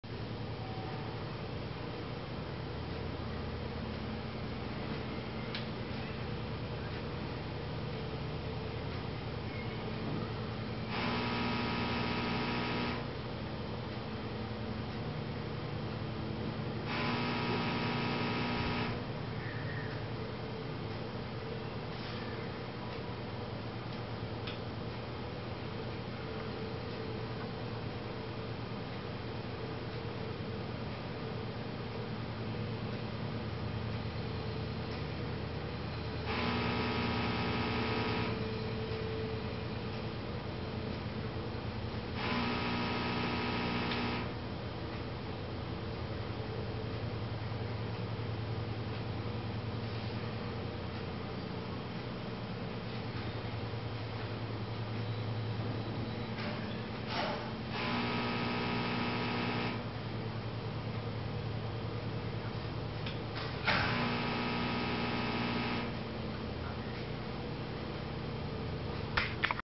Gastrotrich close up
A short video of a Gastrotrich seen under a higher magnification (450x) light microscope.